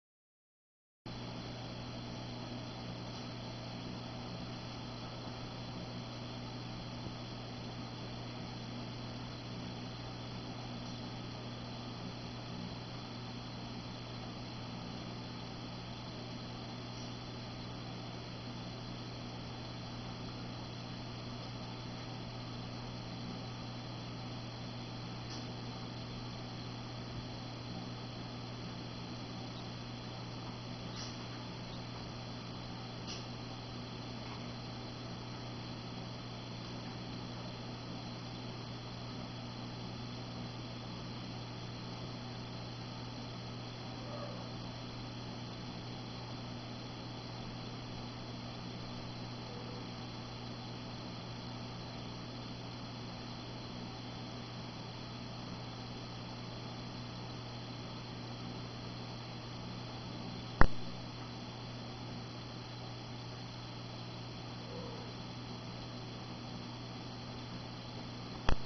Blue Crayfish eating
Crayfish eating a algae wafer.